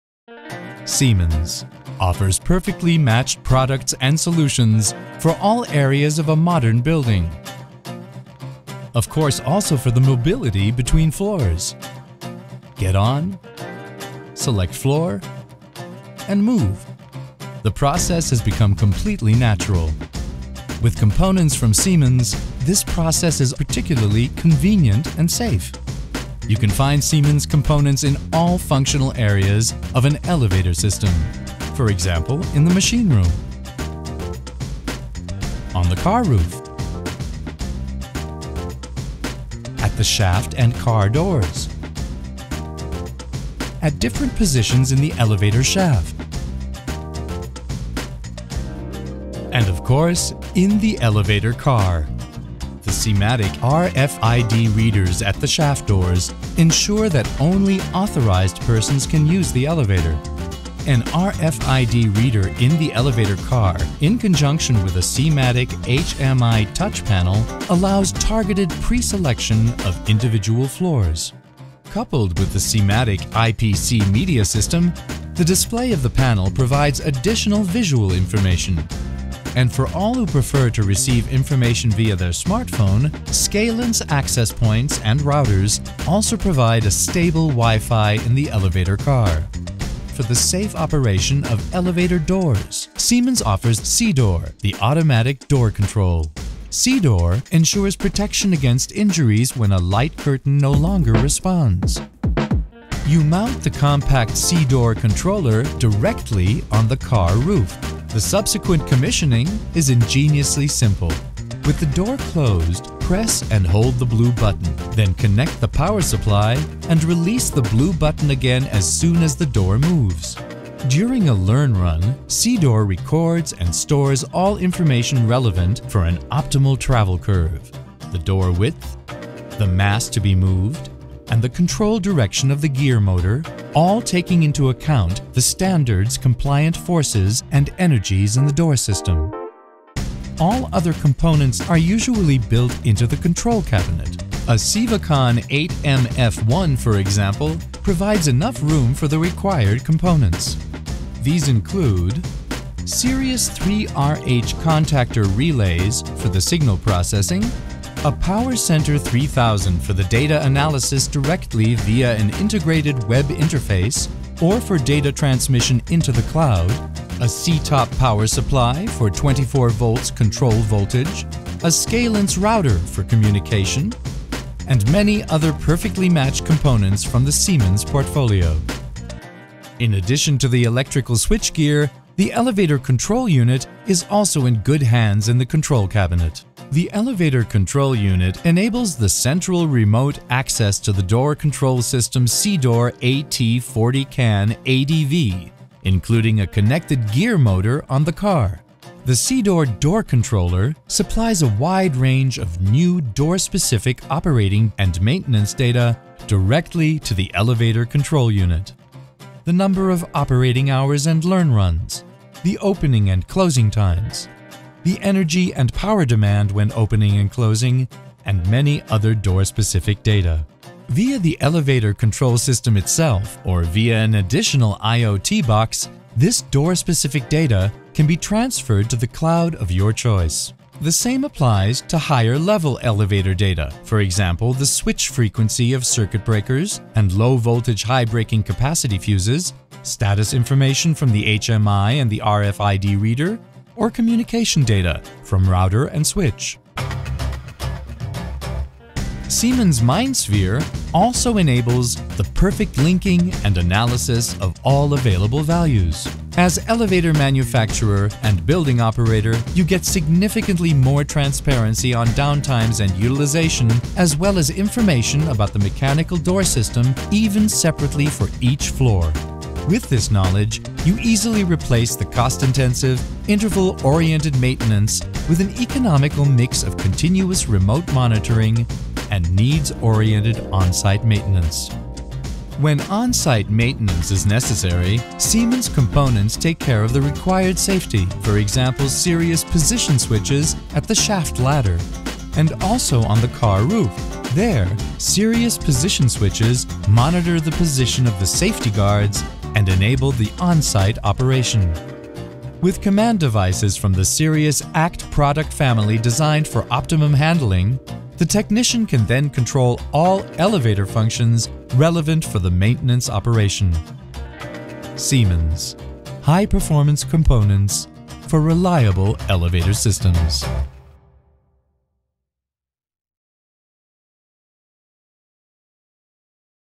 Automation and digitization solutions for elevators and escalators
Visualization and simulation of elevator and escalator data in the building. Siemens components enable safe and convenient operation. We show solutions for door control (SIDOOR), the media system, the cloud and maintenance. Siemens provides powerful components for reliable elevator systems, with the help of which important applications in the building infrastructure are digitized. The communication-enabled components enable remote access. As an elevator manufacturer and building operator, you gain significantly more transparency about downtimes and utilization.
With this knowledge, you can conveniently replace cost-intensive interval-oriented maintenance with an economical mix of continuous remote monitoring and demand-oriented on-site maintenance.